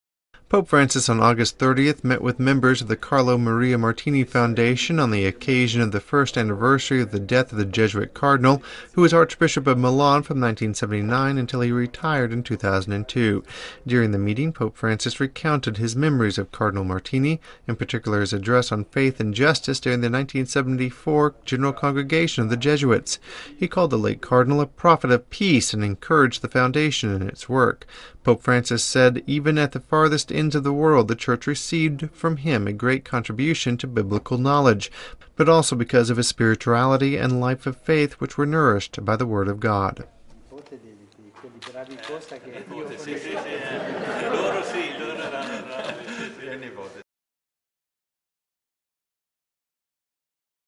Pope Francis praises Cardinal Martini
Francis on August 30th met with members of the Carlo Maria Martini Foundation on the occasion of the first anniversary of the death of the Jesuit cardinal, who was archbishop of Milan from 1979 until he retired in 2002. During the meeting, Pope Francis recounted his memories of Cardinal Martini, in particular his address on faith and justice during the 1974 General Congregation of the Jesuits. He called the late Cardinal a prophet of peace, and encouraged the Foundation in its work. Pope Francis said even at the farthest ends of the world, the Church received from him a great contribution to biblical knowledge, but also because of his spirituality and life of faith, which were nourished by the Word of God.